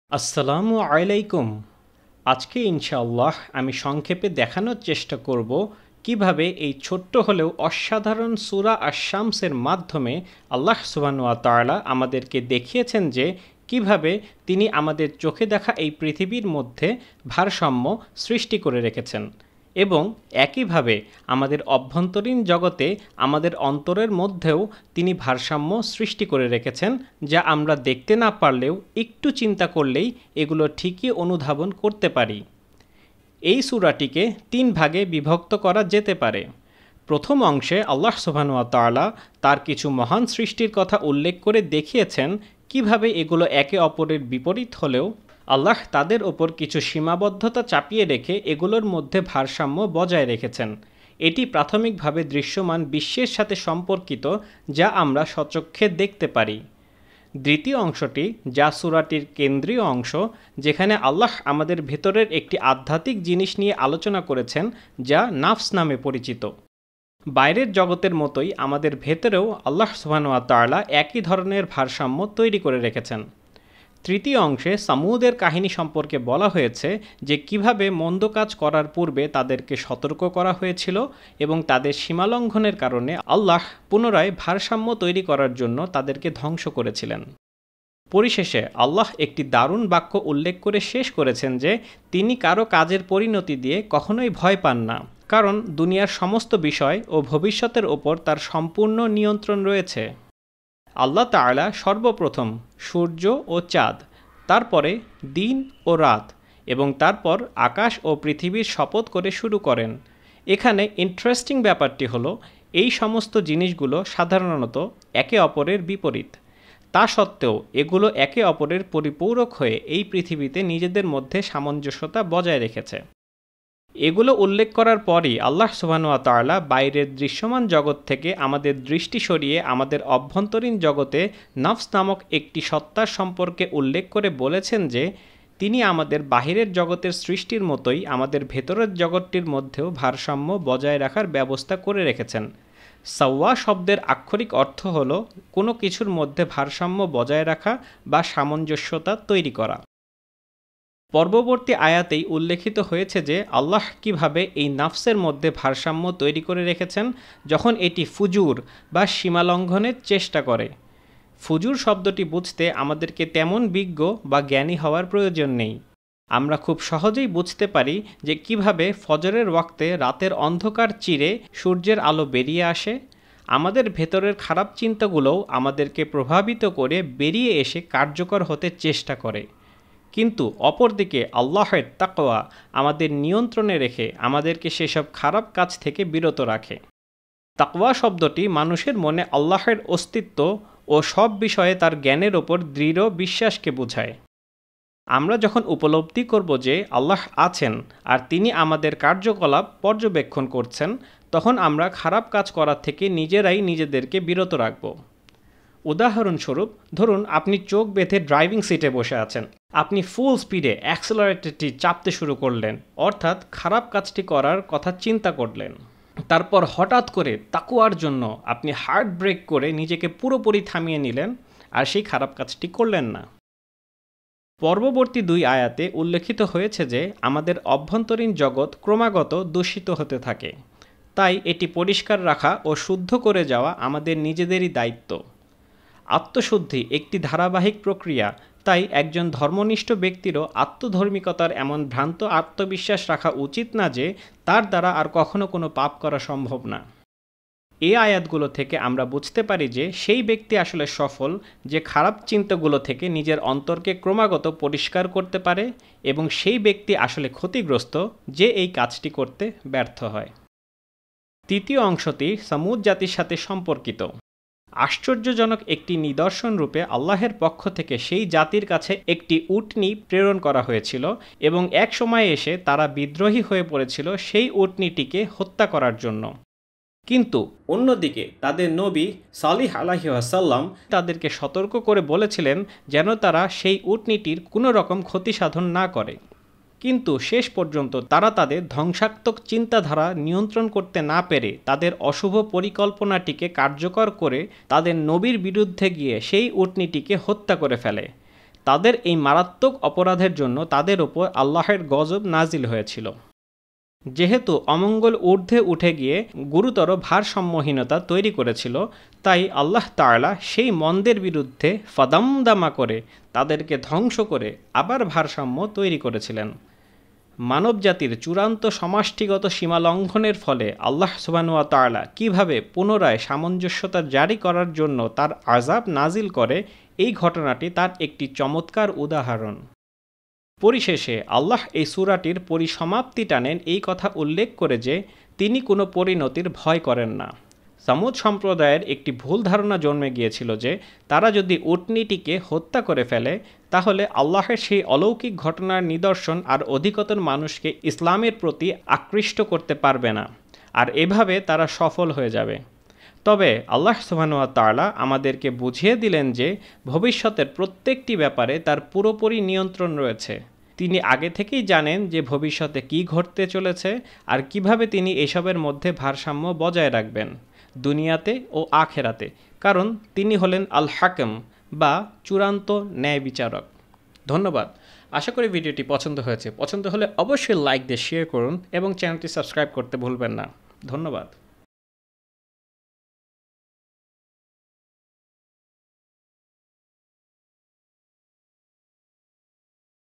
সূরা আস-শামস এর সামঞ্জস্যতা
সূরা শামস এর তাফসির - এই ছোট্ট হলেও অসাধারণ সূরা আস-শামস এর মাধ্যমে আল্লাহ সুাহানাহু ওয়া তা’আলা আমাদেরকে দেখিয়েছেন যে কিভাবে তিনি আমাদের চোখে দেখা এই পৃথিবীর মধ্যে ভারসাম্য সৃষ্টি করে রেখেছেন এবং একইভাবে আমাদের অভ্যন্তরীণ জগতে, আমদের অন্তরের মধ্যেও তিনি ভারসাম্য সৃষ্টি করে রেখেছেন যা আমরা দেখতে না পারলেও একটু চিন্তা করলেই এগুলো ঠিকই অনুধাবন করতে পারি।
এই সূরাটিকে তিন ভাগে বিভক্ত করা যেতে পারে:
প্রথম অংশে আল্লাহ সুবহানাহু ওয়া তা’আলা তার কিছু মহান সৃষ্টির কথা উল্লেখ করে দেখিয়েছেন, কিভাবে এগুলো একে অপরের বিপরীত হলেও, আল্লাহ্ তাদের উপর কিছু সীমাবদ্ধতা চাপিয়ে রেখে এগুলোর মধ্যে ভারসাম্য বজায় রেখেছেন।
দ্বিতীয় অংশটিতে, আল্লাহ আমাদেরকে দেখিয়েছেন  যে বাইরের জগতের মতোই আমাদের ভেতরেও আল্লাহ সুবহানাহু ওয়া তা’আলা একই ধরণের ভারসাম্য তৈরি করে রেখেছেন।
তৃতীয় অংশে সামুদের কাহিনী সম্পর্কে বলা হয়েছে যে কীভাবে মন্দ কাজ করার পূর্বে তাদেরকে সতর্ক করা হয়েছিল এবং তাদের সীমালঙ্ঘনের কারণে আল্লাহ আজওয়াল পুনরায় ভারসাম্য তৈরি করার জন্য তাদেরকে ধ্বংস করেছিলেন।
পরিশেষে, আল্লাহ একটি দারুণ বাক্য উল্লেখ করে শেষ করেছেন যে তিনি কারো কর্মের পরিণতি দিয়ে কখনই ভয় পান না কারন দুনিয়ার সমস্ত বিষয় ও ভবিষ্যতের উপর তাঁর সম্পূর্ণ নিয়ন্ত্রণ রয়েছে।